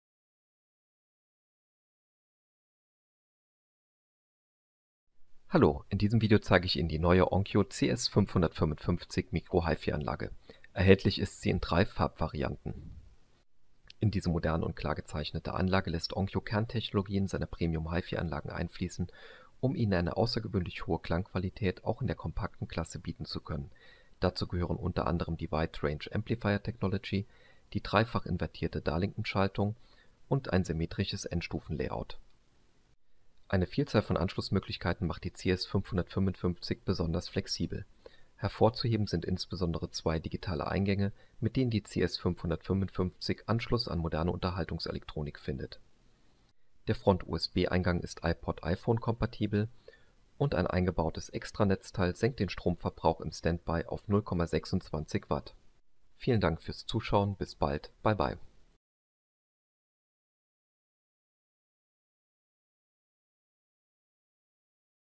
ONKYO CS-555 Vorschau (CD HiFi Mini System iPod iPhone, iPhone5 ready über USB)
Preview zum neuen CS-555 CD HiFi Minisytem - eine Mikroanlage mit Verstärker-Technologien aus dem High-End Bereich. Features: High-End Audiotechnologien (WRAT, 3stufige invertierte Darlington-Schaltung, VLSC gegen Impulsrauschen, symmetrisches Verstärkerlayout, kurze Signalwege, Phase-Matching Bass-Verstärkung), Front-Panel USB-Port für iPod/Phone/Flash-Speicher, hochwertige Regallautsprecher mit N-OMF Membran, digitale und analoge Anschlüsse, Subwoofer-Ausgang, u.v.m.